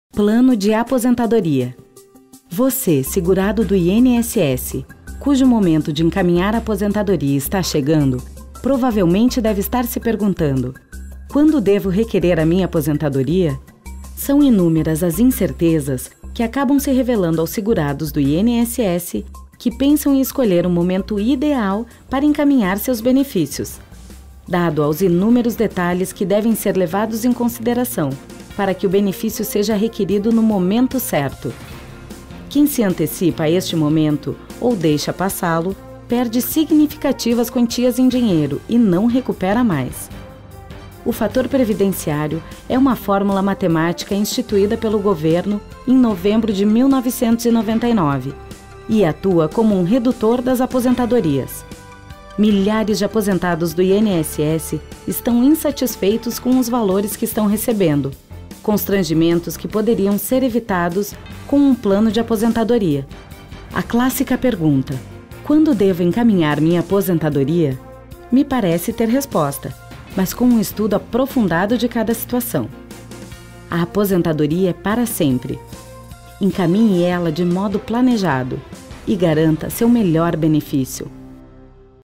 NÃO SE APOSENTE PELO INSS!
Talvez você não tenha se dado conta das complexidades que envolve um encaminhamento de aposentadoria junto ao INSS ainda. Quem sabe você gostaria de conseguir uma aposentadoria mais vantajosa. Sem sombra de dúvida você precisa de um Plano de Aposentadoria. Não se aposente pelo INSS sem antes PLANEJAR a sua aposentadoria. Mais cedo ou mais tarde você vai sentir na pele a importância do que estou te falando.